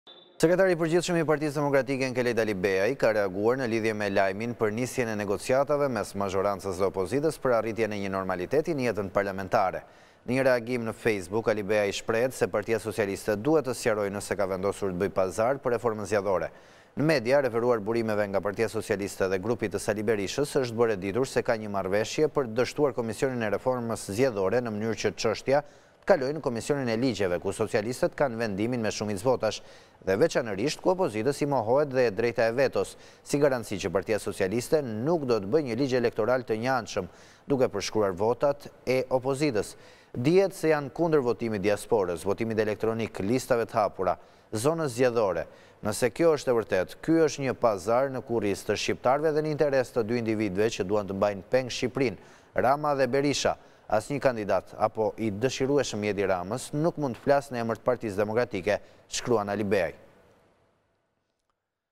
NEWS24 TV / Negociatat? Alibeaj: Askush nuk mund të flasë në emër të PD
Negociatat? Alibeaj: Askush nuk mund të flasë në emër të PD
"PS te sqaroje nese ka vendosur te beje pazar per Zgjedhoren"